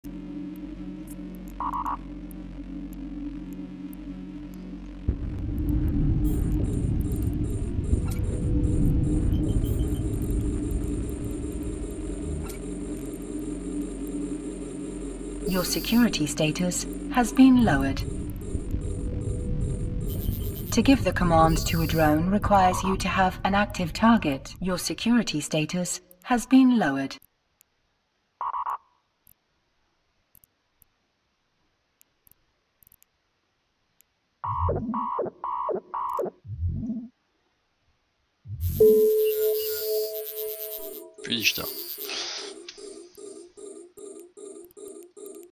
Black OPS Blob !
One proteus catched an ishtar in a chemical labratory in low sec.
Two sin jumped in !